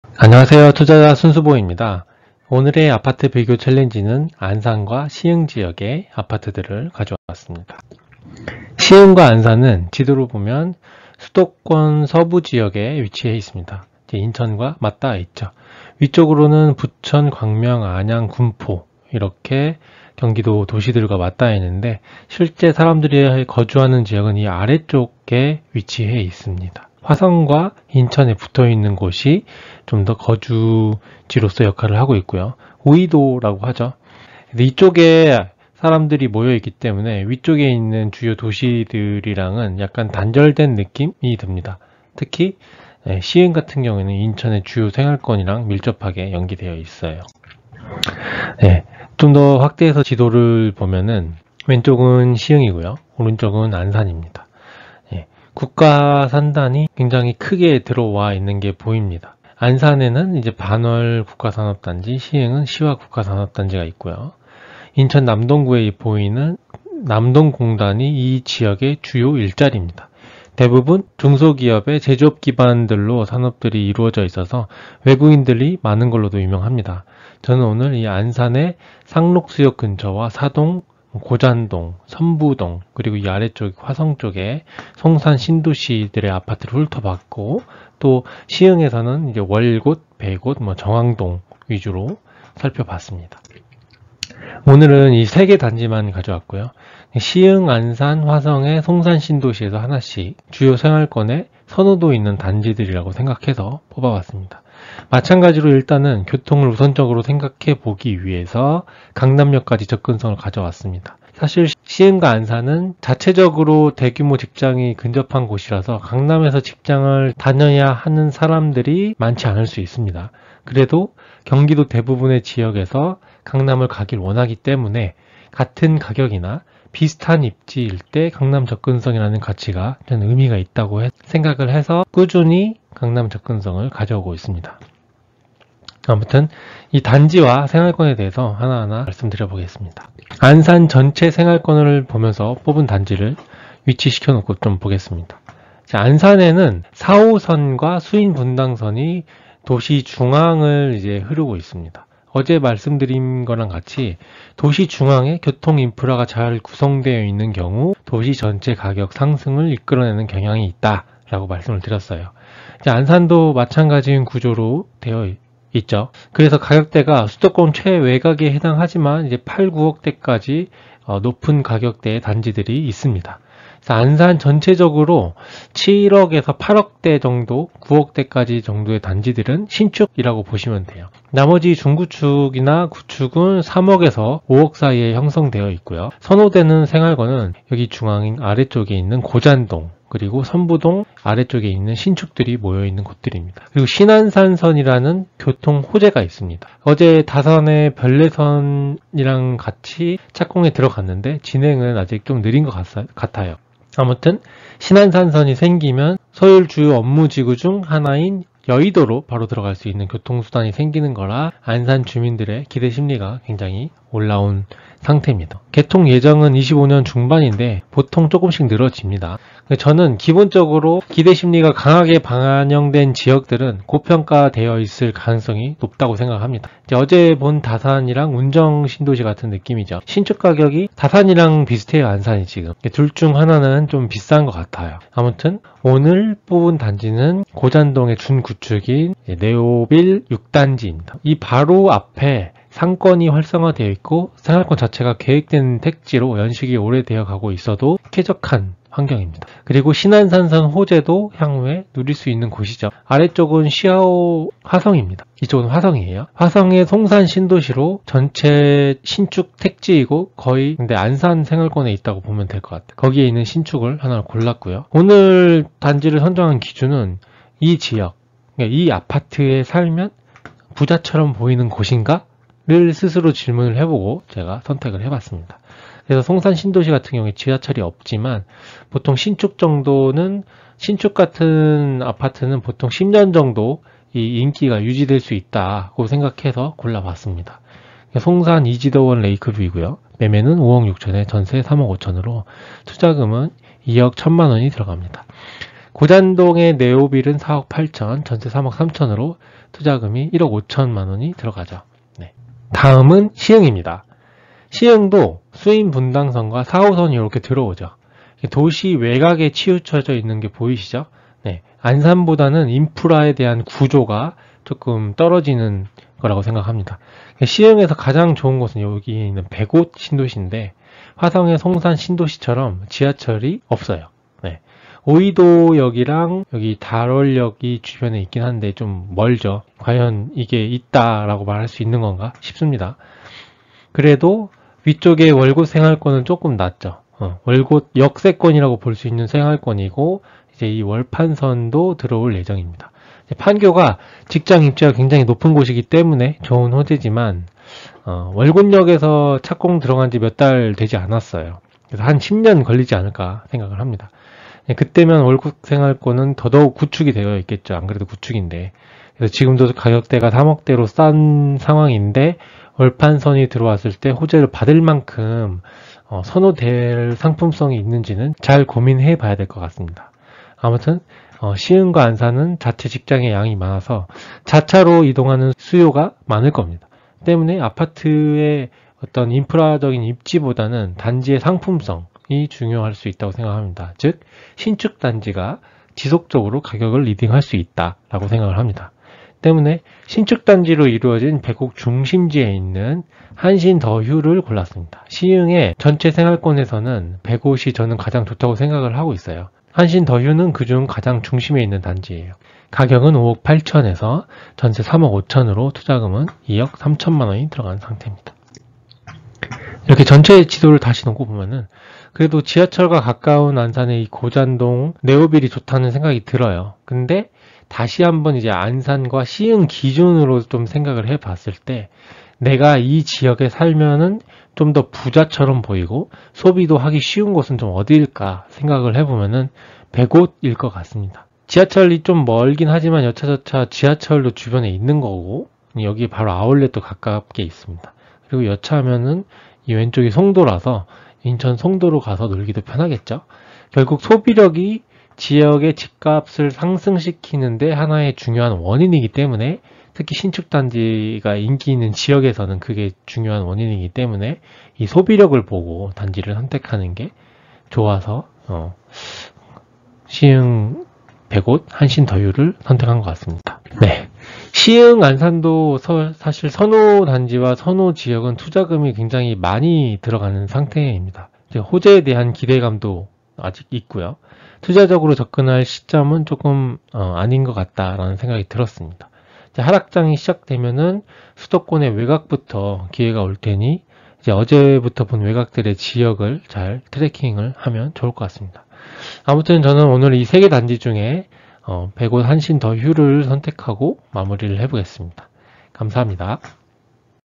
[순수생각] 아파트 비교 챌린지 #20 서울 수도권 (안산 시흥)
[00']31 고잔동 네오빌6단지 4.8 / 3.3 (투자금 1.5억)
[18']34 송산그린시티이지더원레이크뷰 5.6 / 3.5 (투자금 2.1억)
[18']35 배곧동 시흥배곧한신더휴 5.8 / 3.5 (투자금 2.3억)